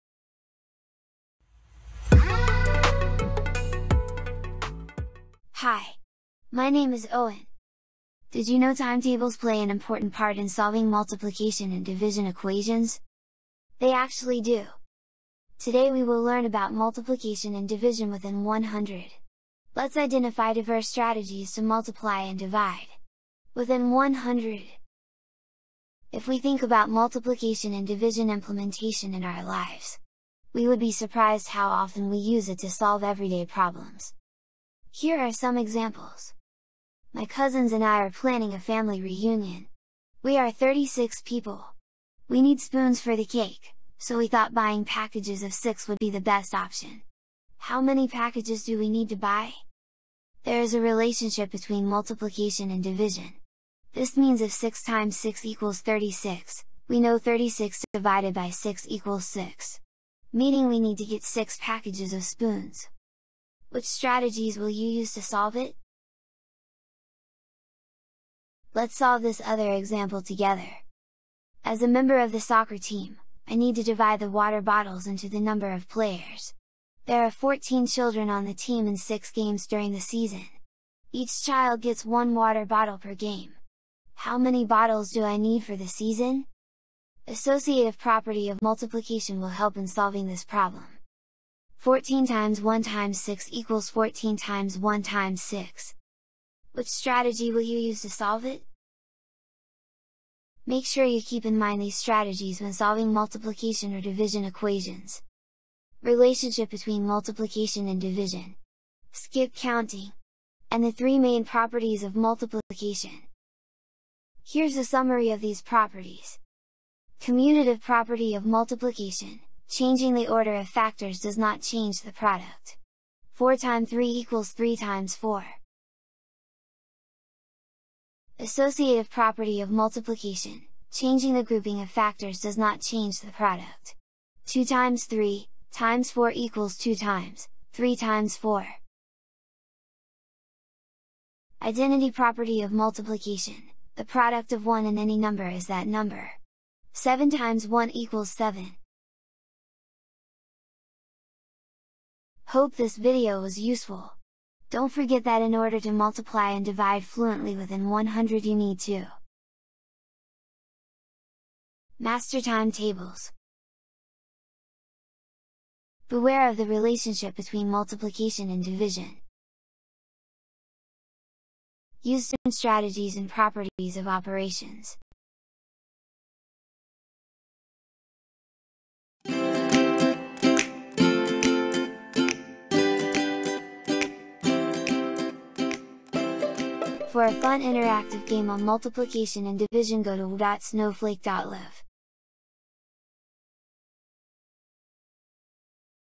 Multiplication and Division Within 100
This video shows different strategies to multiply and divide within 100.

Please visit Snowflake.live to access lesson plans, fun interactive games and other educational resources.